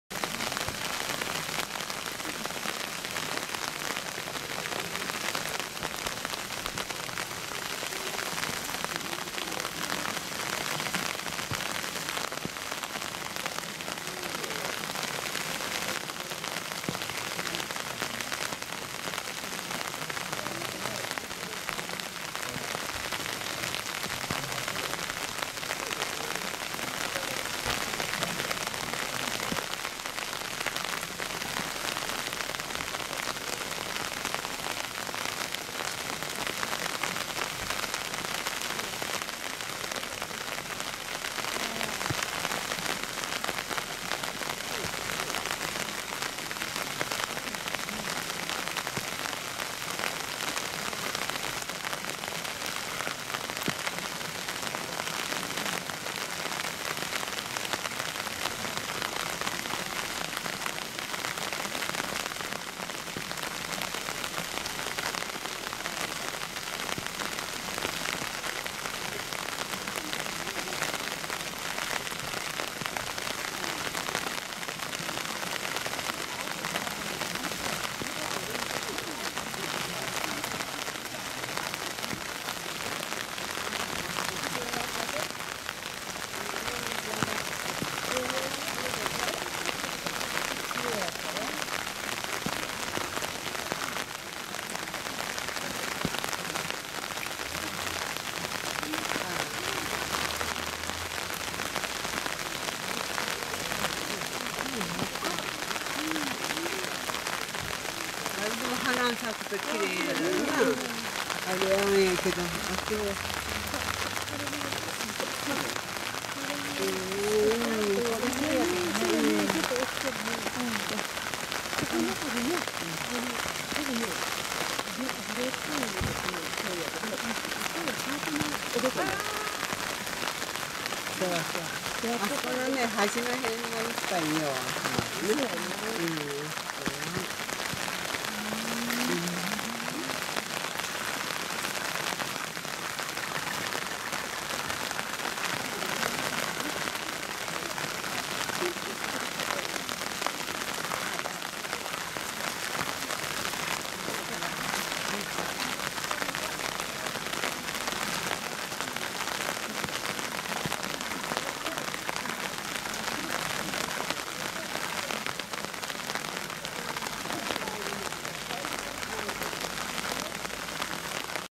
The small beautiful pond
This pond is called Monet's pond.
Location: Gifu prefecture